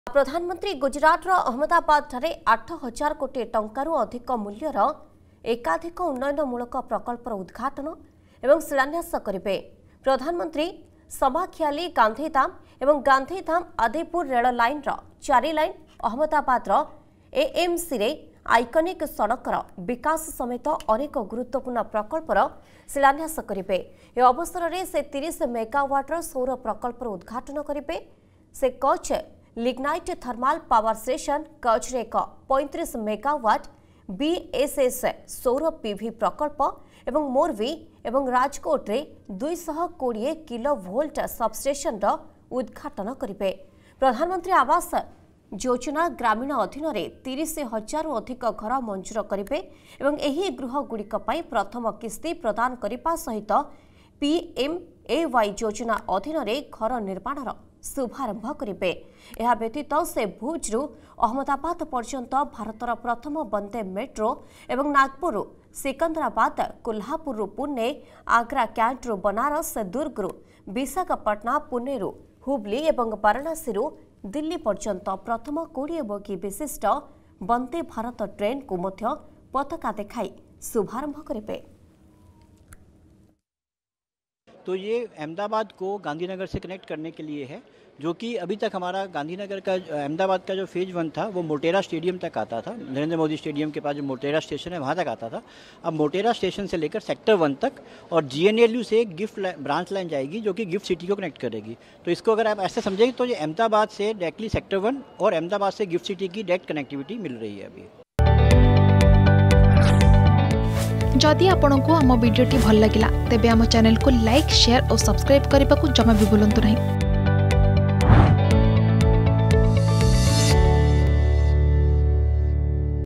ଅହମ୍ମଦାବାଦରେ ପ୍ରଧାନମନ୍ତ୍ରୀ ମୋଦି || Narendra Modi
ପ୍ରଧାନମନ୍ତ୍ରୀ ଗୁଜରାଟର ଅହମ୍ମଦାବାଦଠାରେ ୮ ହଜାର କୋଟି ଟଙ୍କାରୁ ଅଧିକ ମୂଲ୍ୟର ଏକାଧିକ ଉନ୍ନୟନମୂଳକ ପ୍ରକଳ୍ପର ଉଦଘାଟନ ଏବଂ ଶିଳାନ୍ୟାସ କରିବେ।